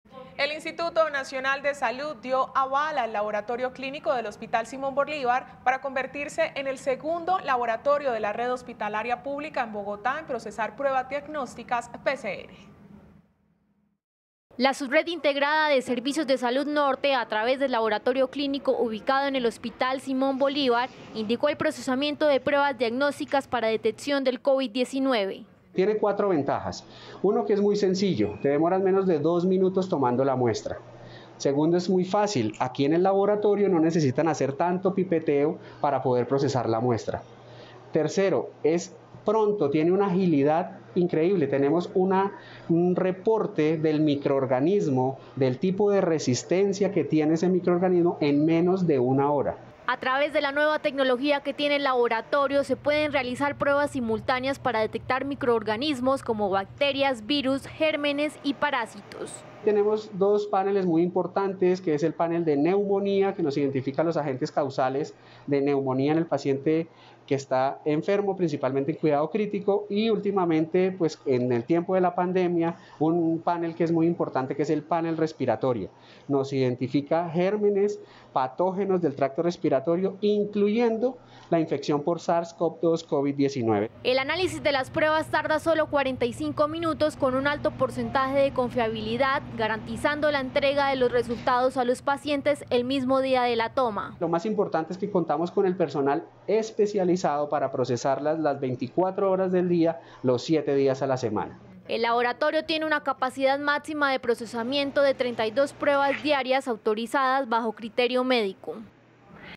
Nuevo laboratorio para procesar pruebas de COVID-19
NoticiasCapital | Bogotá cuenta con un nuevo laboratorio equipado con un sistema ágil y preciso para detectar el virus del COVID-19. El análisis de las pruebas solo tarde 45 minutos.